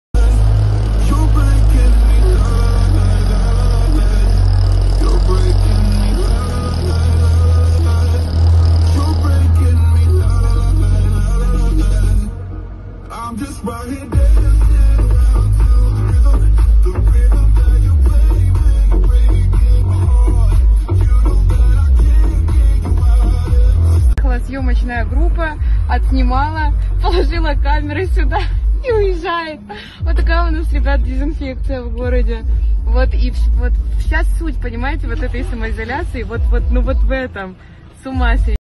Имитацию дезинфекции улиц удалось заснять жительнице Пятигорска
Источник: Telegram-канал «MDK»